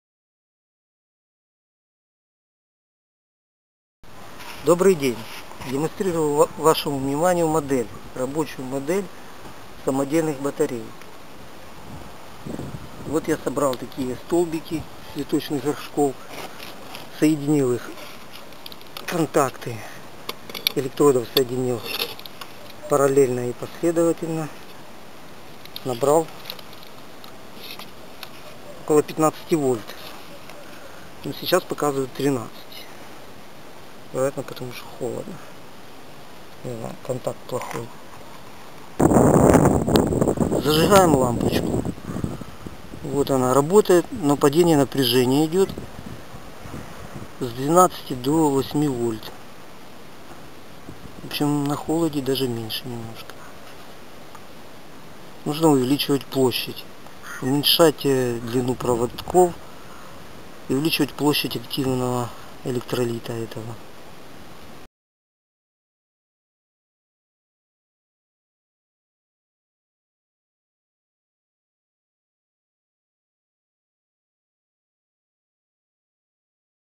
Енергія без мереж. Autonomous energy
Незалежне джерело електричного струму можливо  зробити самостійно кожному, використовуючи при цьому доступні, підручні матеріали.Нема сумніву в тому, що у давнину 1000 років тому, люди мали можливість користуватись і виготовляти подібні пристрої, генерації електричного струму.